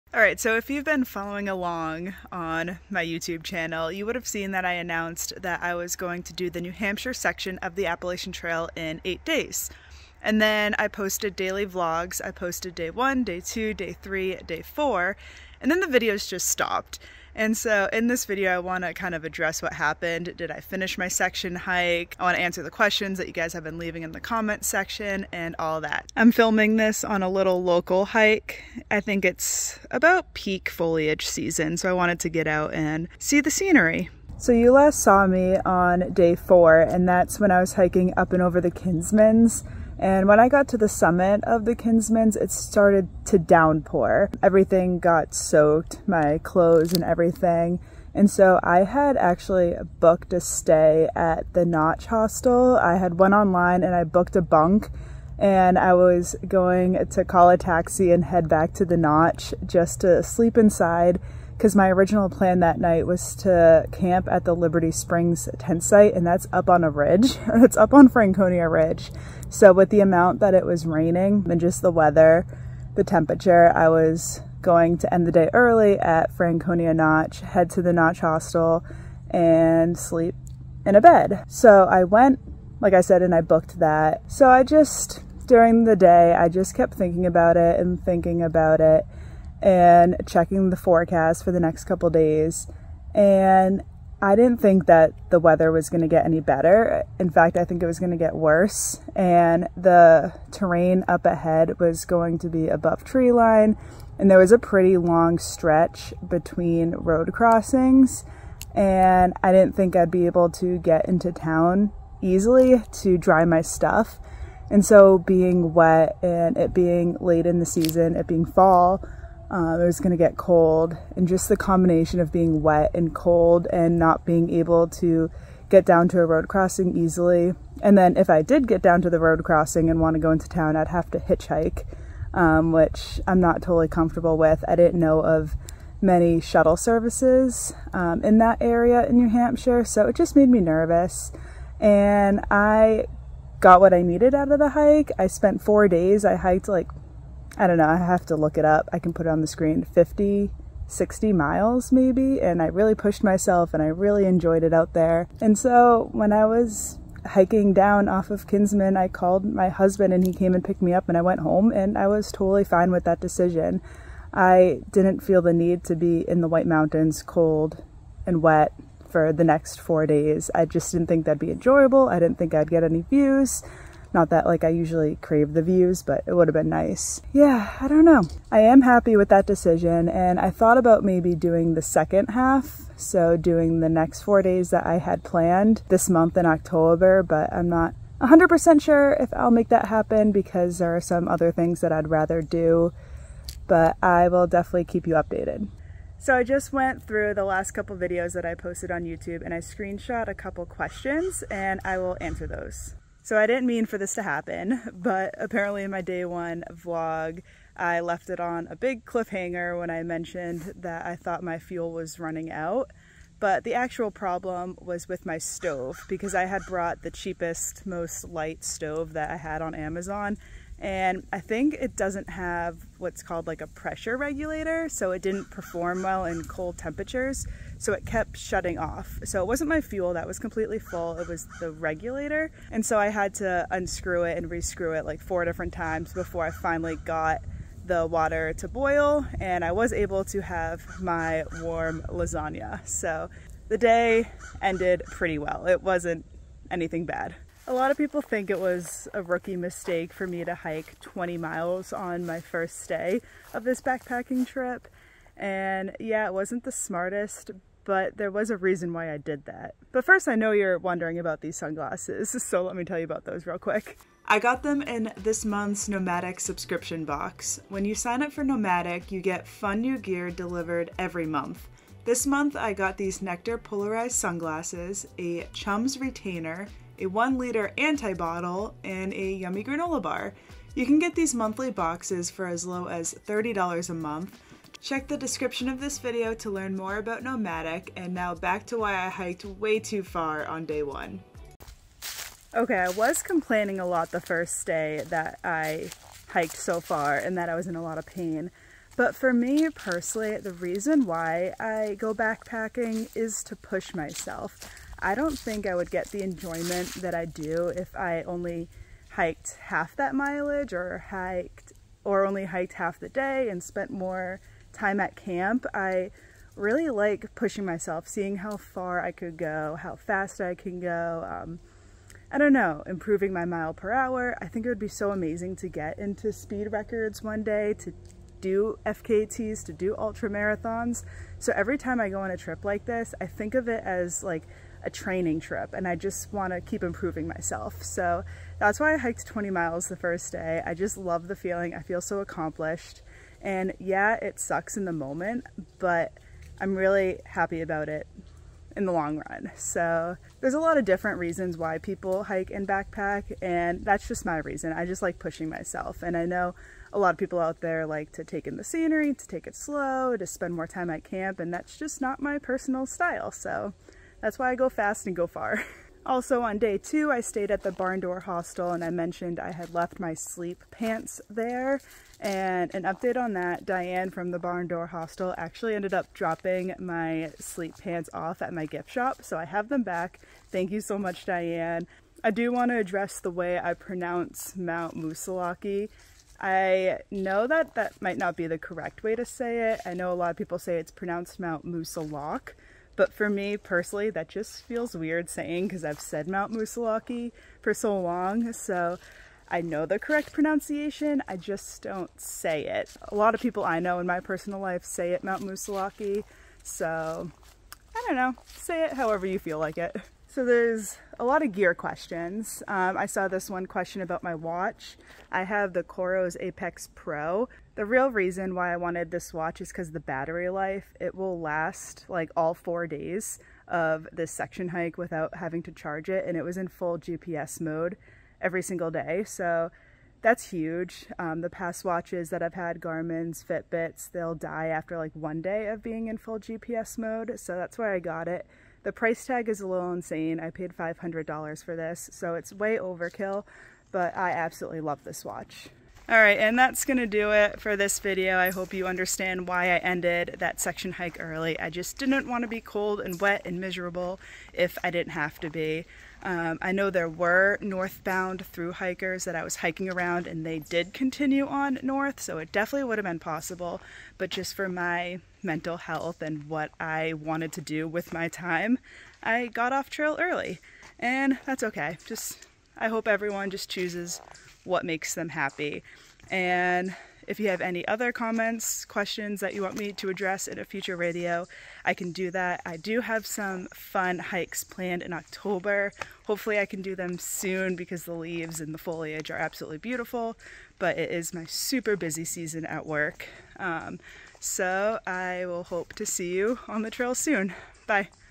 Why I Stopped My Appalachian Trail Section Hike Vlogs...
I finally had some time to film a video and explain why I stopped my New Hampshire section hike vlogs.
I explain what happened on the trail and answer some comments that were left under my videos.

Coros Apex Pro Watch